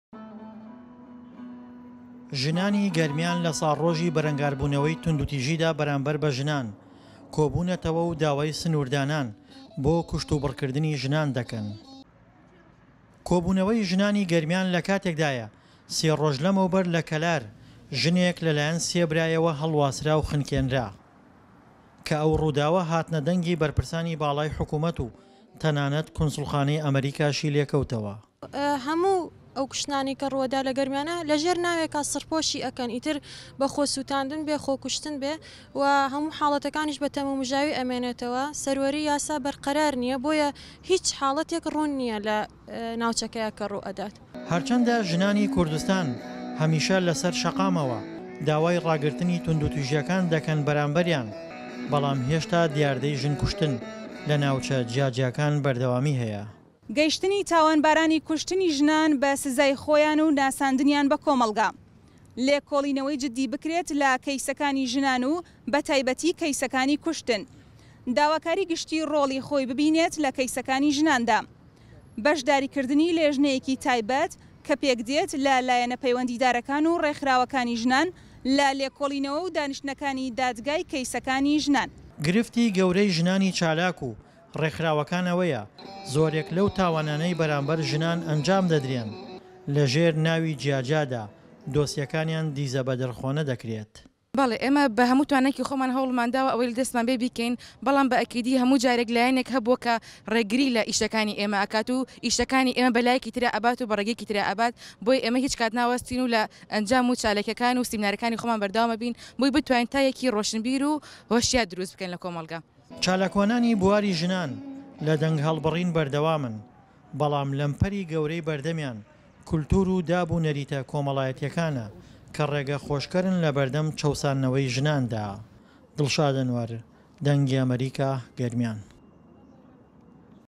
TUNDÛTÎJÎYA DIJÎ JINAN LI HERÊMA KURDISTANÊ
Jinên Germîyanê li Herêma Kurdistanê dijî tundûtîjîya ser jinan çalakî lidarxistin û daxwaza wekhevî û jîyanek aram kirin. Jibo agahî û pêzanînên pirtir kerem bikin li wê vîdyoyê binêrin.